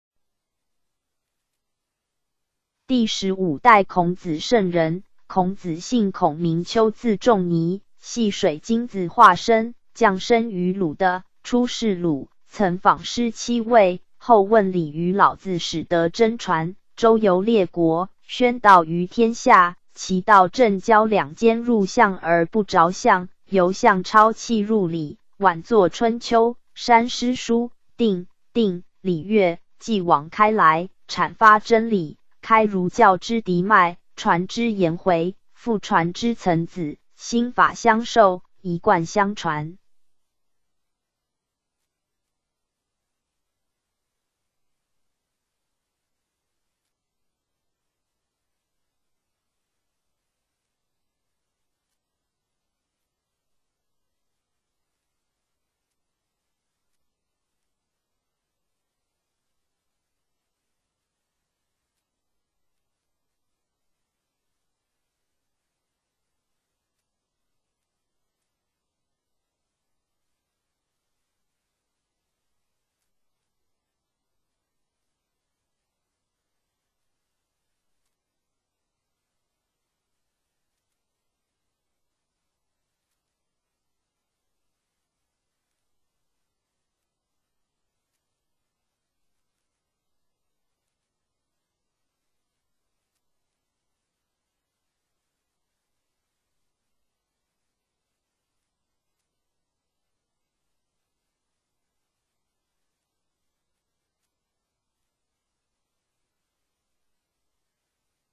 六十四代祖師第15代#孔子聖人：孔子姓孔名丘字仲尼，係水精子化身 ， 降生於魯地，初仕魯，曾訪師七位，後問禮於老子始得真傳，周遊列國，宣道於天下，其道政教兩兼入象而不著象，由象超氣入理，晚作春秋、
刪詩書、定(訂)禮樂，繼往開來，闡發真理，開儒教之嫡脈，傳之顏回，復傳之曾子，心法相授，一貫相傳